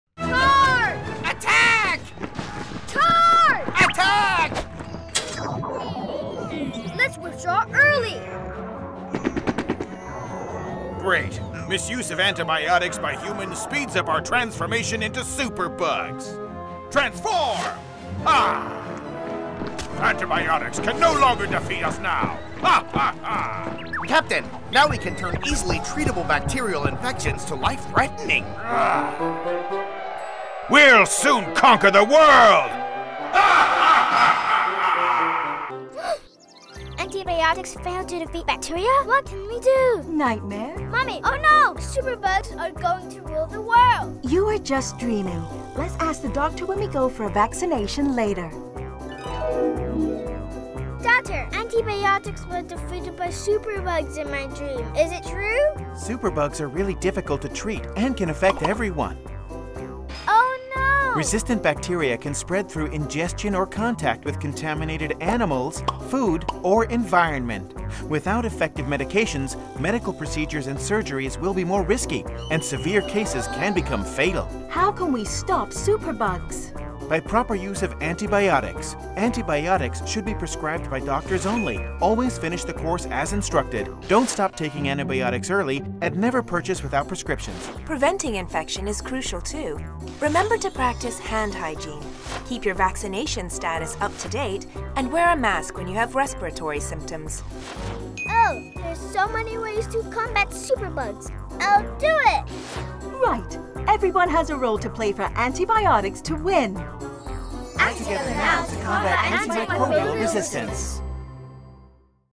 Act Together Now to Combat Antimicrobial Resistance (Full version)
Antimicrobial resistance (AMR) occurs when microorganisms (e.g. bacteria, fungi and viruses etc.) resist previously effective antimicrobials. Antibiotics are the most widely known antimicrobials used to treat bacterial infections. When bacteria develop resistance to antibiotics, antibiotics are no longer effective to wipe out these “Superbugs”. This causes infections becoming more difficult to treat, and makes medical procedures and surgery more risky. To prevent “Superbugs”, we need to use antibiotics properly, maintain personal hygiene and keep vaccination up-to-date.